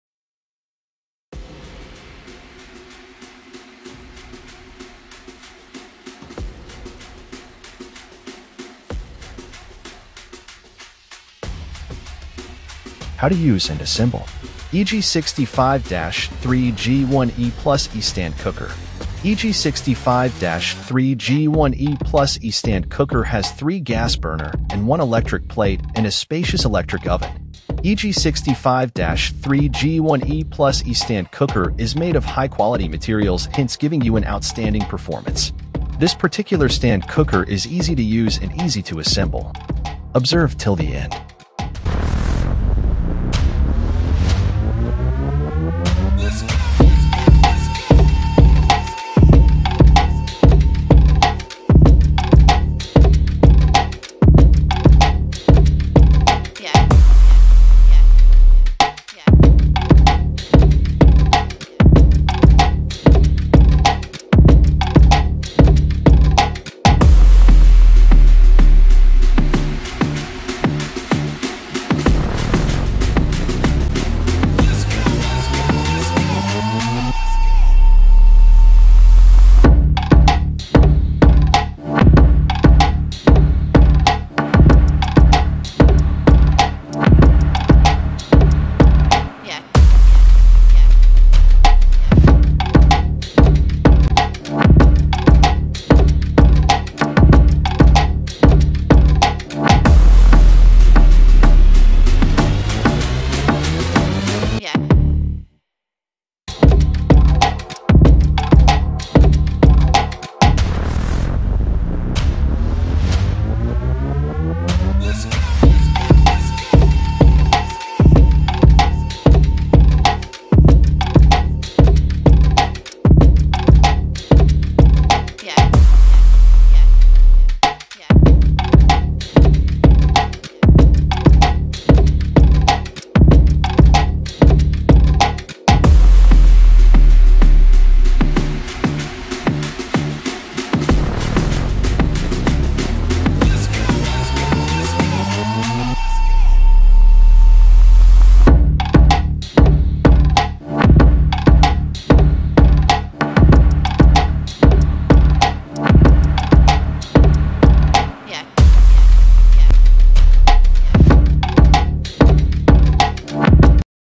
How to use and assemble the Eurochef EG65-3G1E+E stand cooker
Eurochef is a major electronics company in kenya that deals in Mutimedia systems and Kitchen appliances. The EG65-3G1E+E is a cooker with 3 gas burners and 1 hot plate and an electric oven.

Available on our website and shops countrywide.